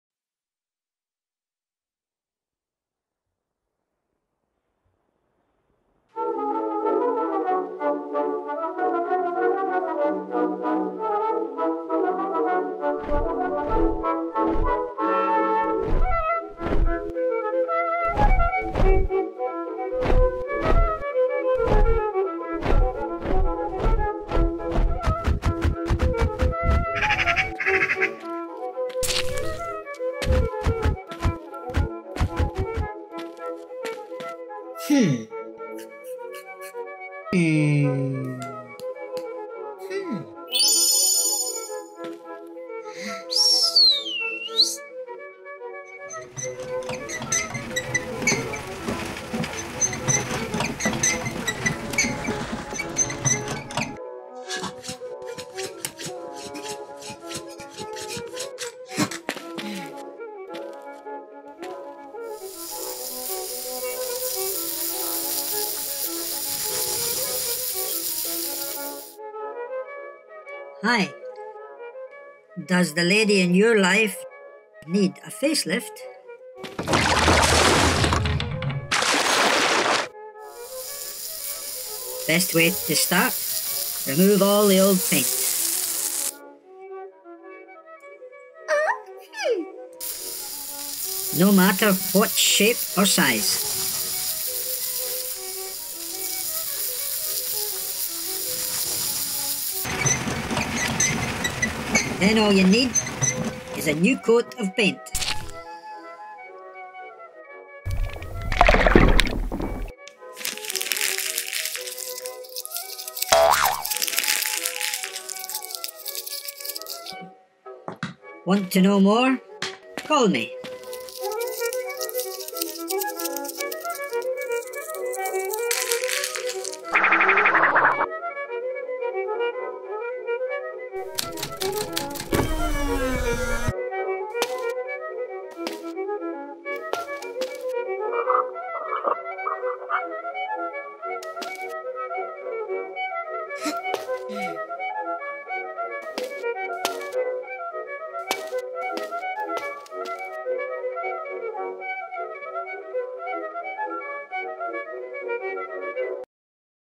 Stop motion movie b BRAVO
I created this advert for a company. everything is made by hand.
Programs used. Dragonframe, adobe photoshop, adobe after effects, adobe premiere, adobe audition.
Bjorn´s voice by Bill Mundy